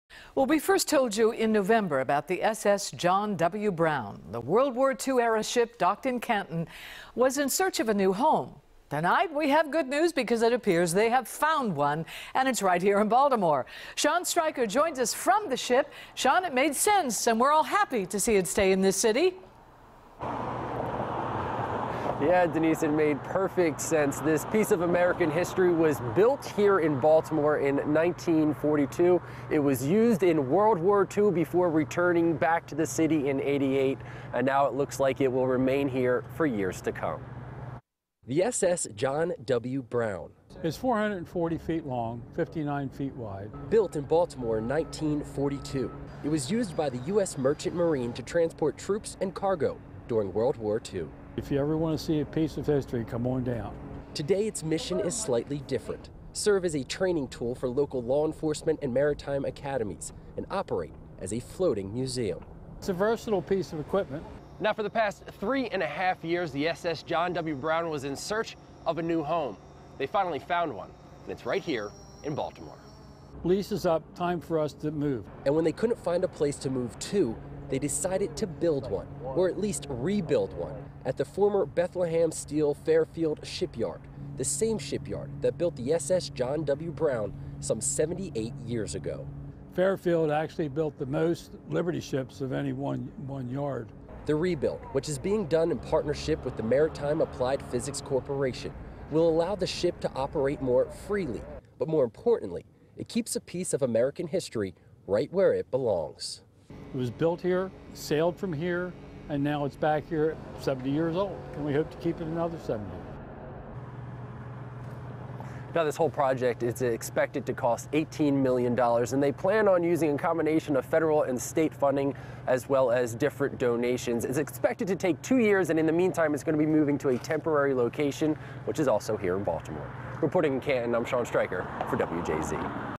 Once In Jeopardy, SS John W. Brown Finds New Home In Baltimore
After several years of uncertainty, a piece of Baltimore history has found a familiar home that will keep it in the city for the foreseeable future.